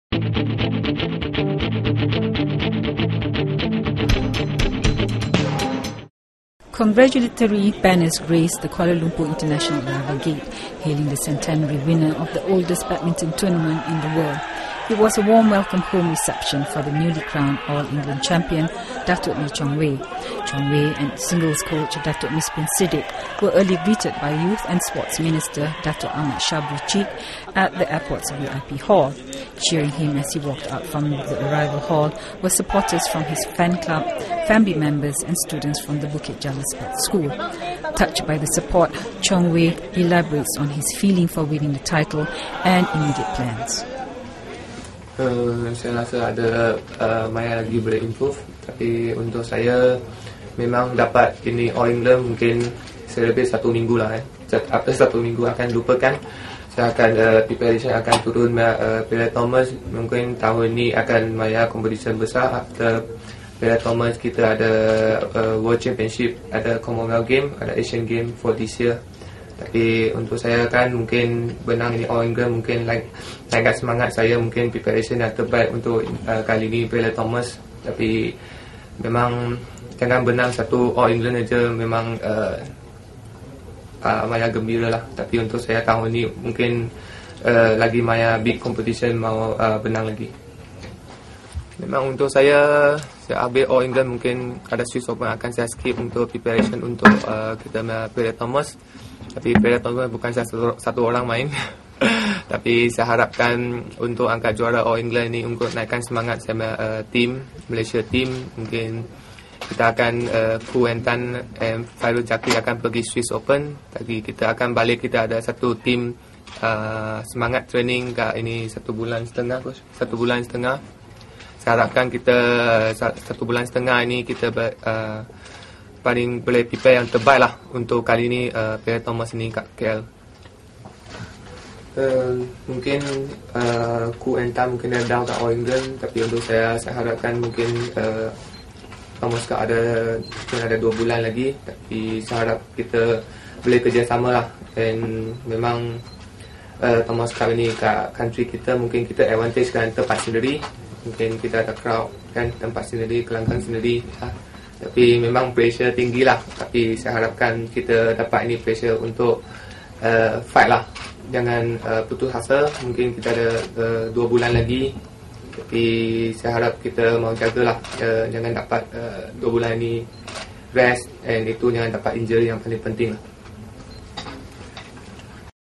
Warm reception for All England champ
Datuk Lee Chong Wei arrived home to a warm welcome at the Kuala Lumpur International Airport on Tuesday. Touched by the reception, the newly crowned All-England champion talked about winning the title and his immediate plans.